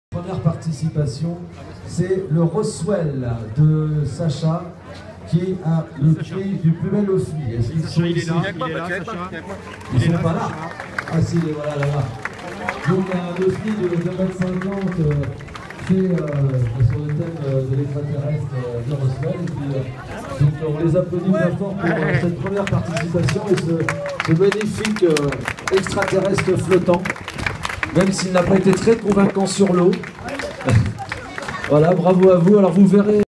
Ofni 2018 SAINT MARTIN DE BRÉHAL une coupe décernée
Le groupe Facebook  """ saint Martin de brehal le nouveau site """ possède sa coupe !!!
malheureusement plus de mémoire au niveau du téléphone !! donc vidéo courte et inachevée ... car il parlait du groupe FB